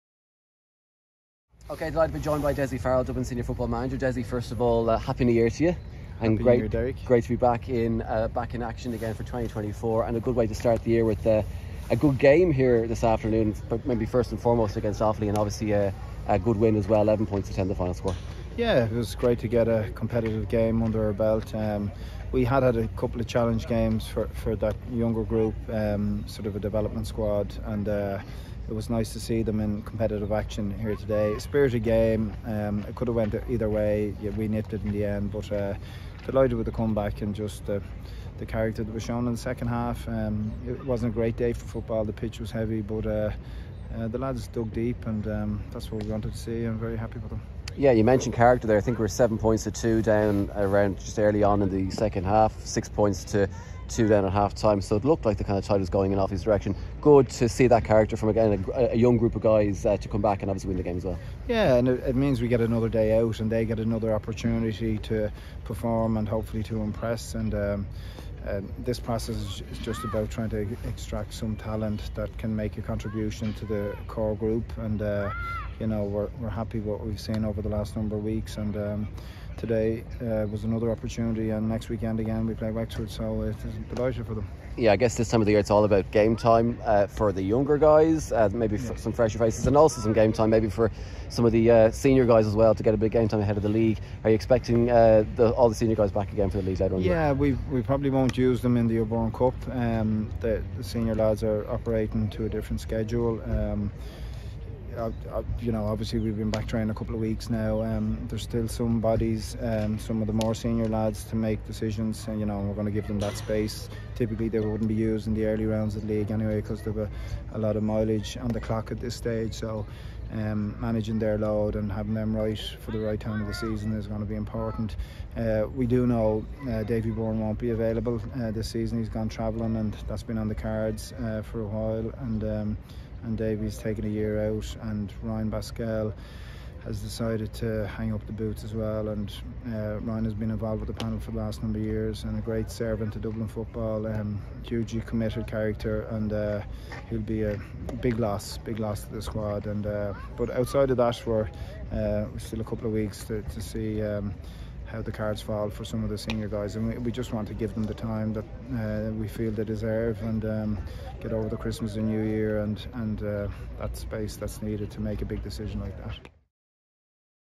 Dessie Farrell speaks to DubsTV following O'Byrne Cup QF win over Offaly
"We're delighted with the comeback & the character that was shown in the second half..."

Dublin Senior Football manager Dessie Farrell spoke to DubsTV after his side's comeback win over Offaly in the Quarter Final of the Dioralyte O’Byrne Cup.